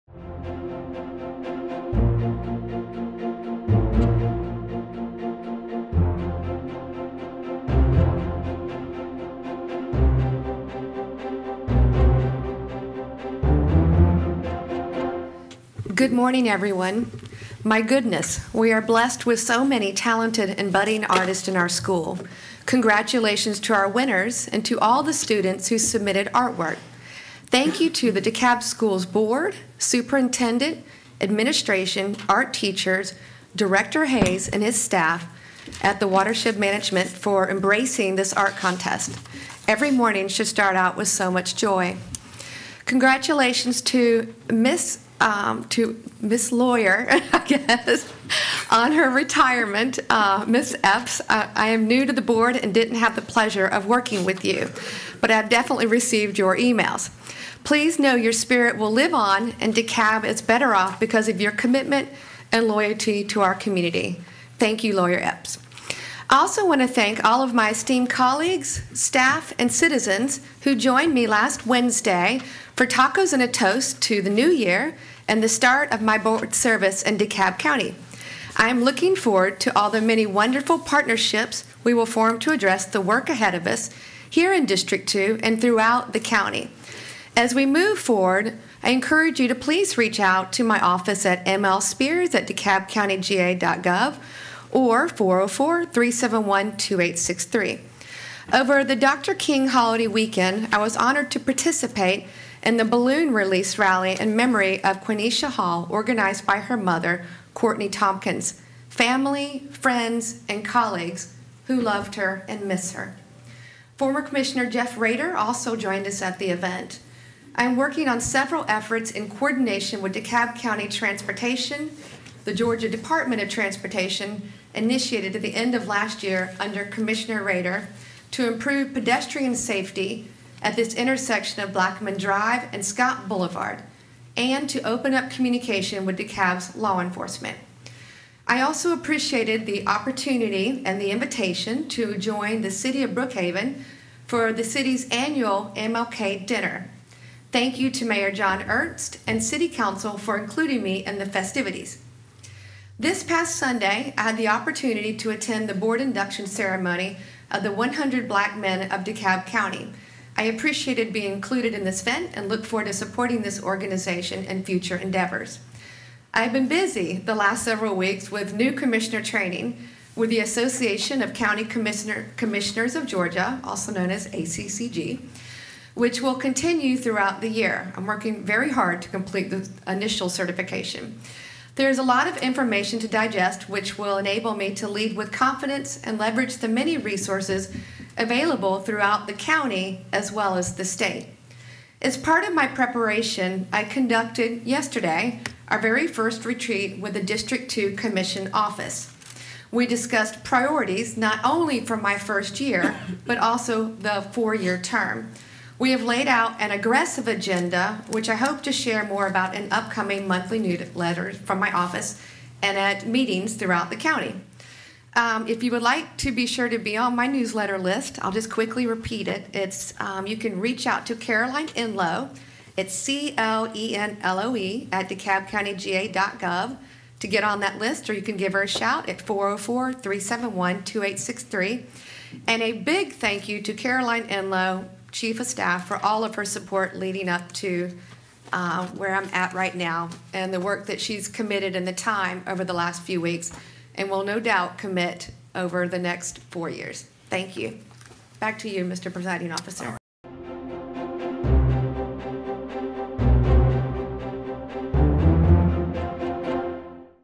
Commissioners' Corner - District 2 Michelle Long Spears 1/24/23
Aired live on DCTV (DeKalb County Television), Channel 23, this video excerpt of the most recent DeKalb County Board of Commissioners' meeting features announcements specific to each commission district.